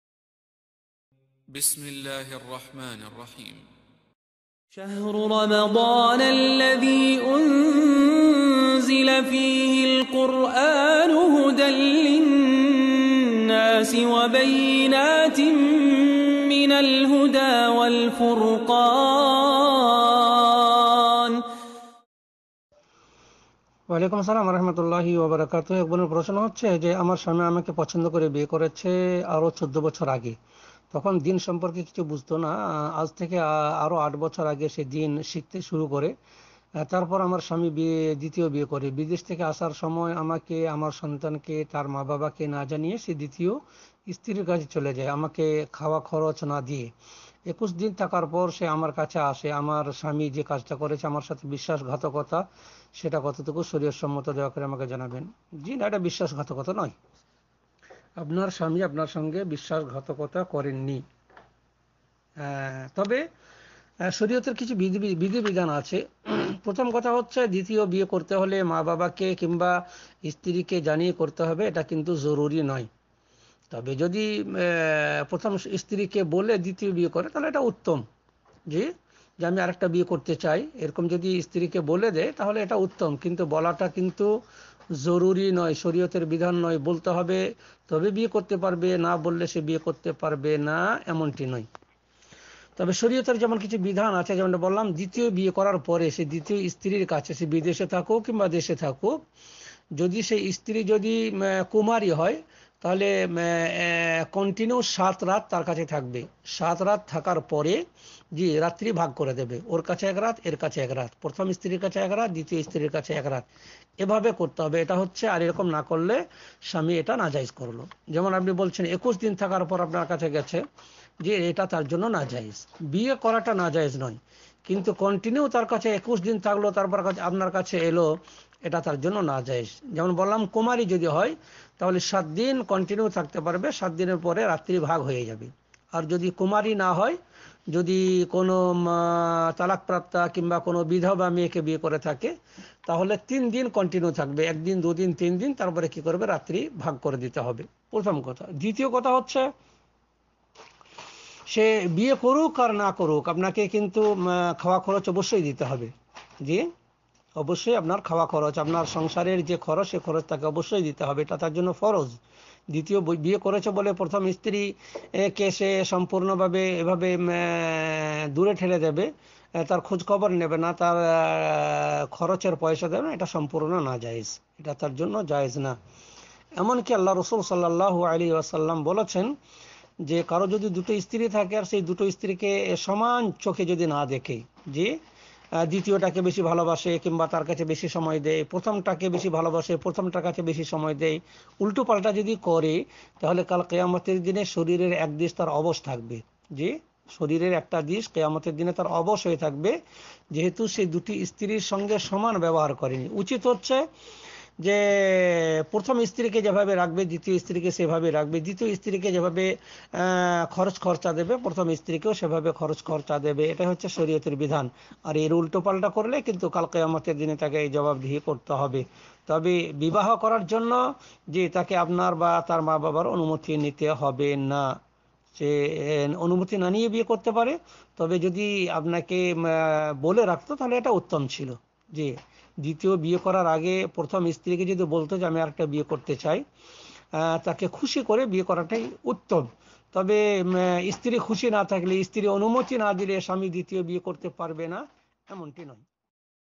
স্বামী না জানিয়ে বিবাহ করেছে এ বিবাহ কি বৈধ? আলোচক শফিউর রহমান।
স্বামী না জানিয়ে বিবাহ করেছে এ বিবাহ কি বৈধ? আলোচক শফিউর রহমান। عليكم ورحمة الله وبركاته